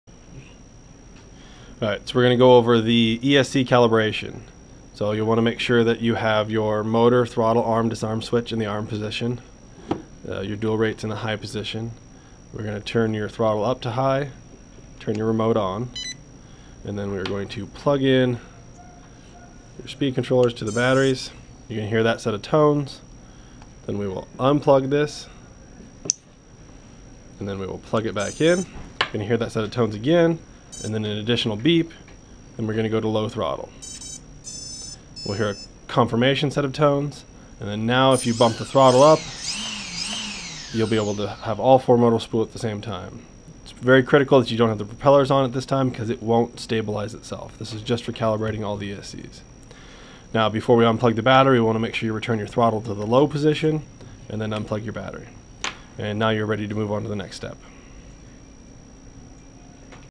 MINDS-i: How to calibrate your ESC's
In this video Levi will show you how to calibrate your ESC's for the MINDS-i Multirotor UAV Kit.

What can you build?